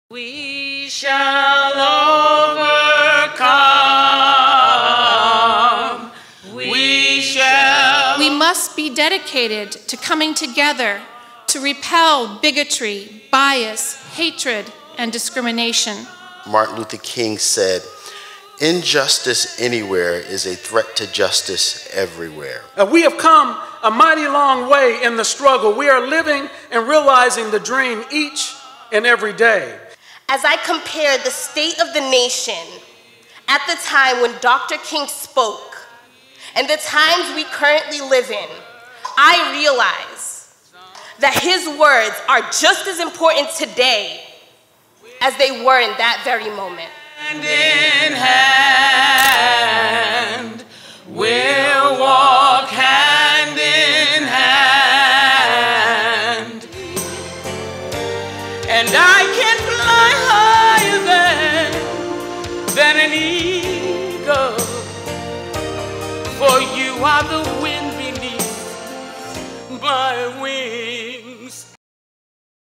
Bristol Community College Dr. Martin Luther King Jr. Community Breakfast 2018 Recap
Recap of the Dr. Martin Luther King, Jr. Community Breakfast 2018